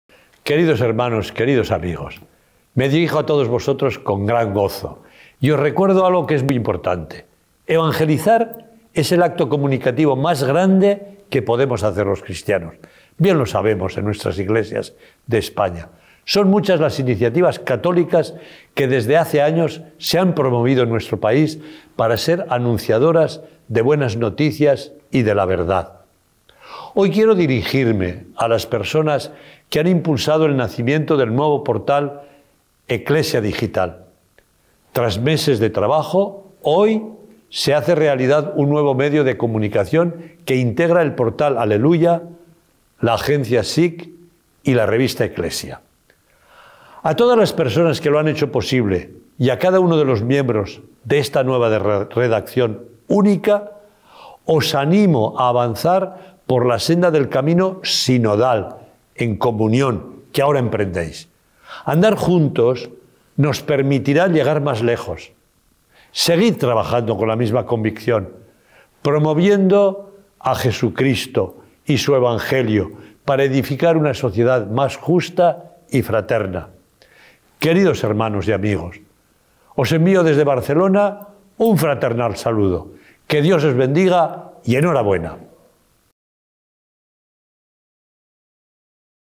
El cardenal Omella anuncia la fusión de los medios digitales de la Conferencia Episcopal
Todos los medios digitales de la Conferencia Episcopal- 'Revista Ecclesia', 'Agencia SIC' y 'Aleluya'- se fusionan en ecclesia.es.